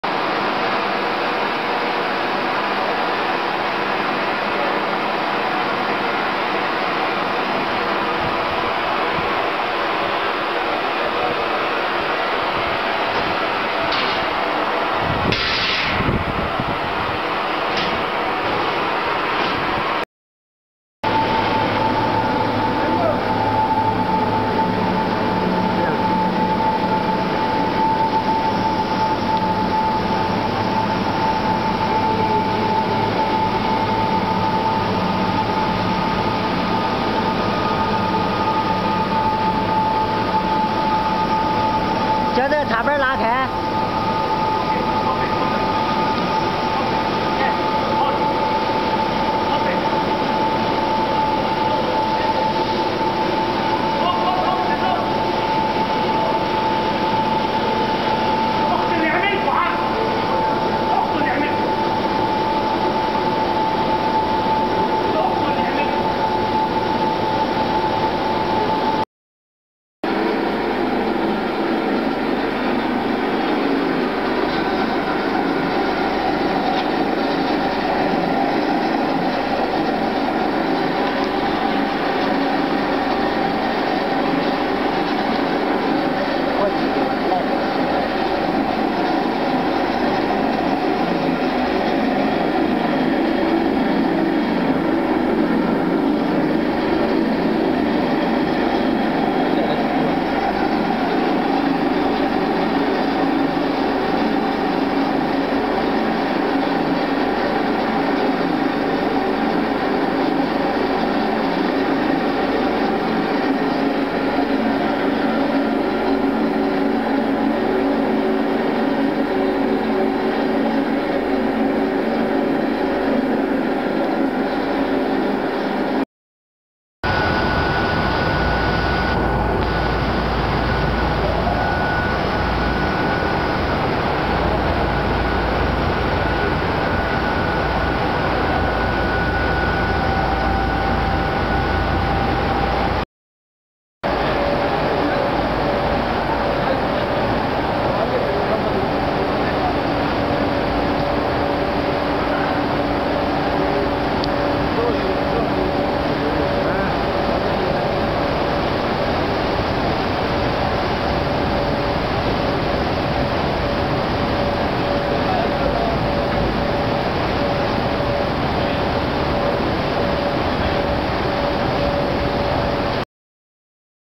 Sesame Production Equipment|Sesame Processing Line|Sesame Oil Plant| Hulling Sesame Production Line
Video is a live shot we took to show off our industrial sesame seed process line in customer factory site.

Pls let me know what capacity need for this new project?